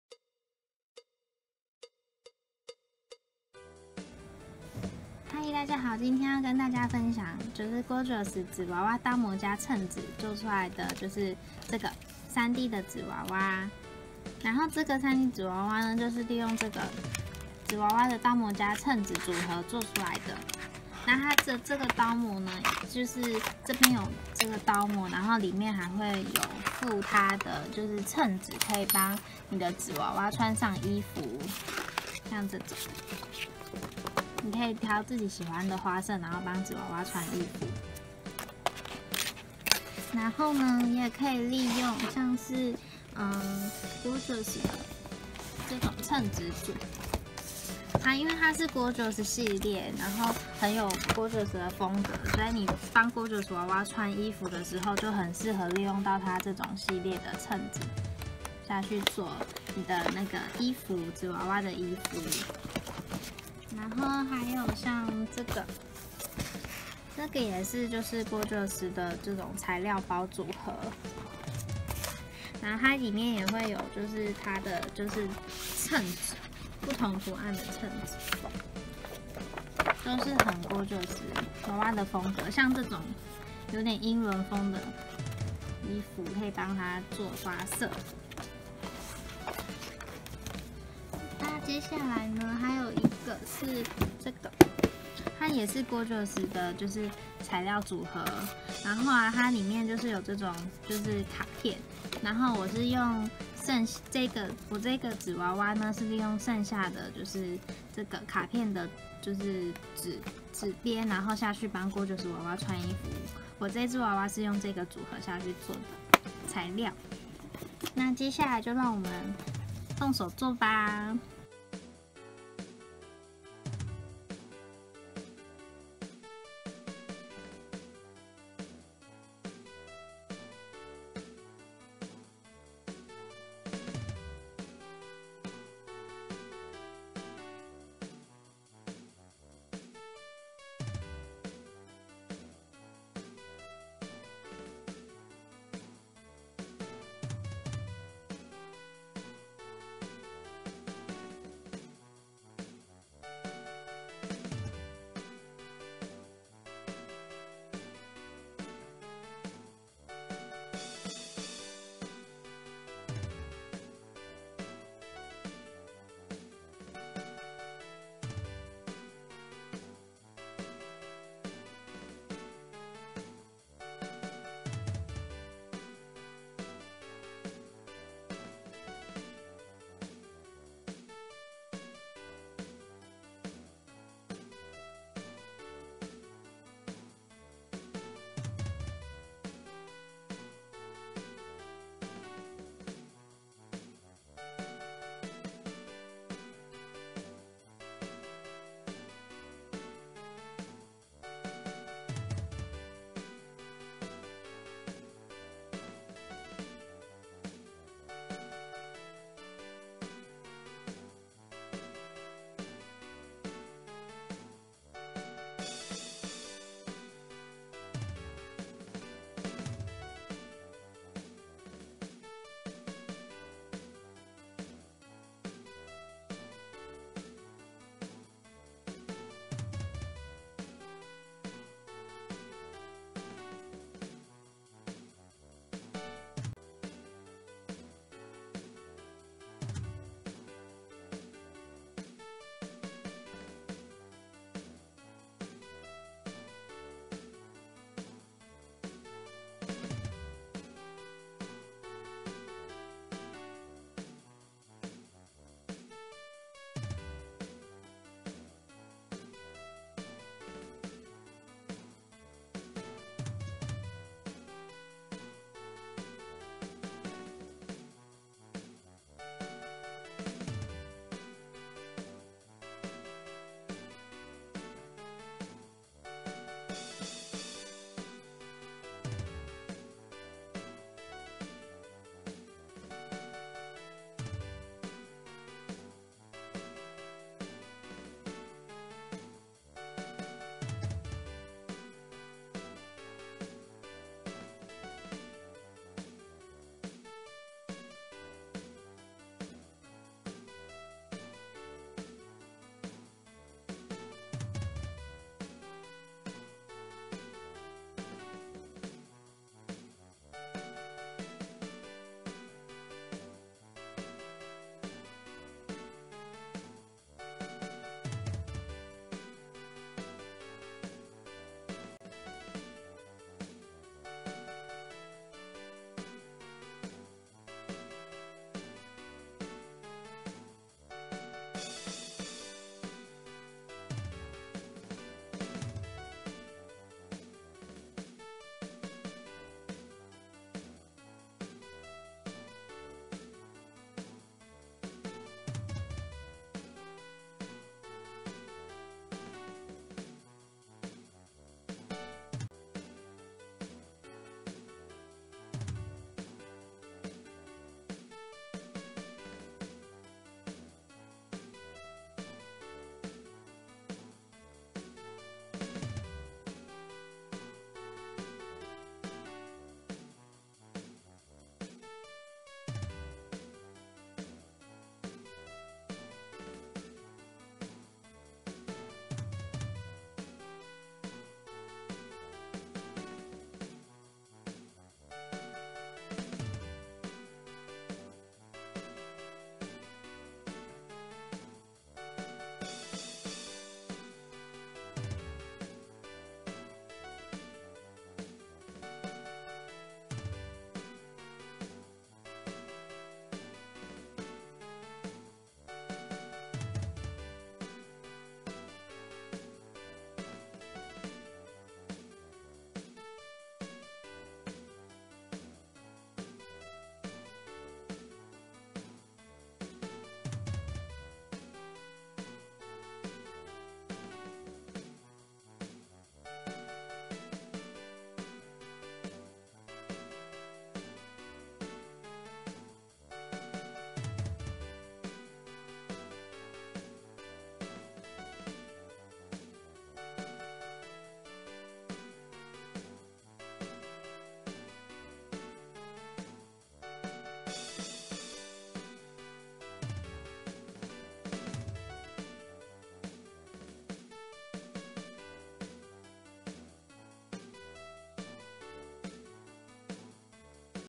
牧莎DIY手藝教室-Gorjuss Dies 紙娃娃刀模示範
Gorjuss Dies 紙娃娃刀模分解步驟示範影片Action🎥

選喜歡的襯紙自己幫Gorjuss娃娃配衣服
可愛Gorjuss服裝由你來搭配💛
你們快來一起玩😚😚

同場推薦工具:
Cosmic Shimmer Glue 無酸膠 30ml